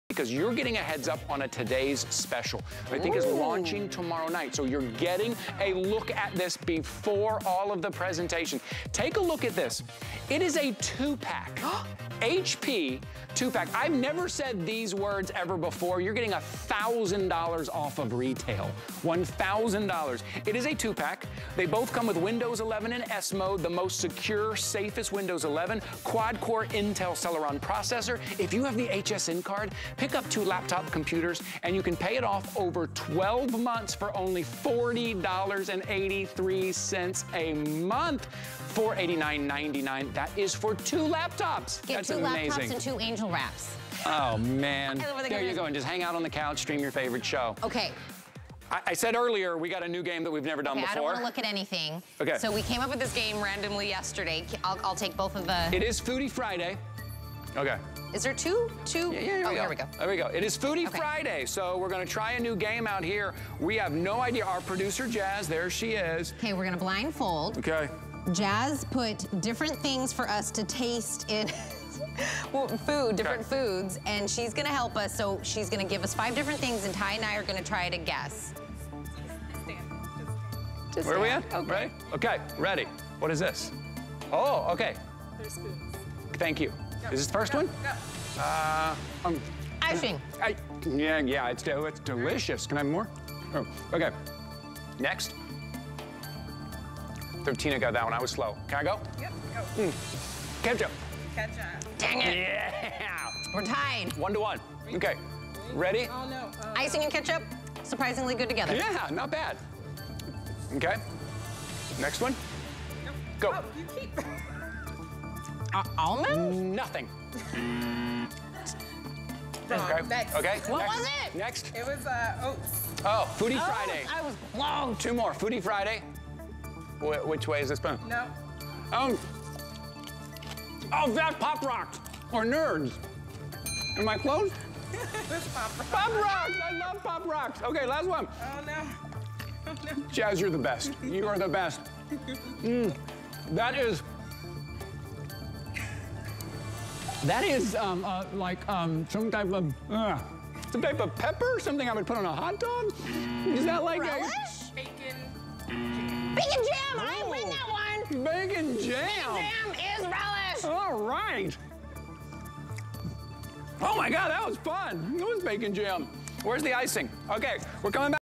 HP 2pack 14" Intel Laptops with MS Office 365 HP Prote...
HP 2pack 14" Intel Laptops with MS Office 365   HP Protection

When you combine superb HP quality with incredible portability and the allnew Windows 11, you get this stunning 14" laptop.

    2 HP Stream 14" laptop
    2 Power adapter
    Documentation

Bundled Goodies

    2 (1)year subscriptions to Microsoft Office 365
    2 PC Lifestyles Software vouchers

    Windows 11 in S mode is a version of Windows 11 that's streamlined for security and performance, while providing a familiar Windows experience. To increase security, it allows only apps from Microsoft Store, and requires Microsoft Edge for safe browsing. To run any program or use other web browsers, you will have the ability to opt out of S mode. If you choose to opt out, you will lose some of the security features S mode provides and you will not be able to switch back.